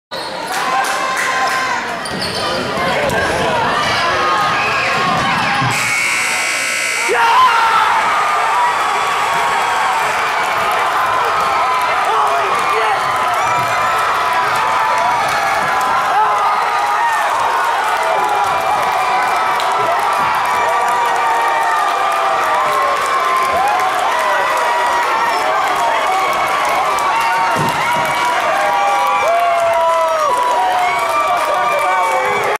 Purcell Marian Buzzer Beater Defeats Georgetown (OH)
Purcell Marian's Carlik Jones buzzer beater vs Georgetown G-Men in first round of Ohio Division III Tournament.